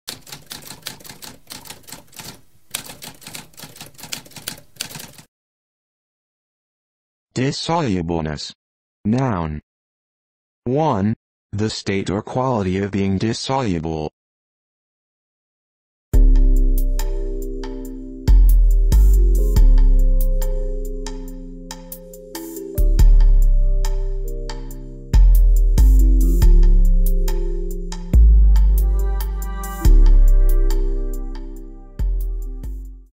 What does dissolubleness mean?
A spoken definition of dissolubleness.

Intro Sound:
Typewriter - Tamskp